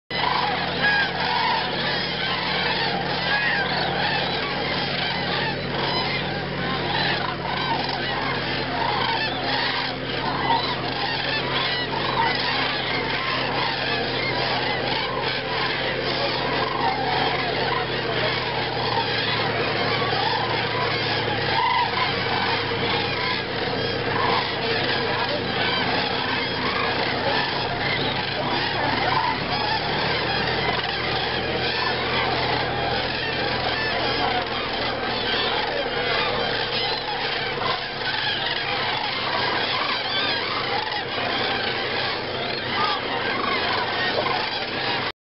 Golden Jackals looking for a meal
2 Golden Jackals in a field surrounded by few thousand Common Cranes at the Hulla Valley Israel ,the 2 jackals waiting for the right moment to hunt one of the cranes around them, usually one of the slow or sick one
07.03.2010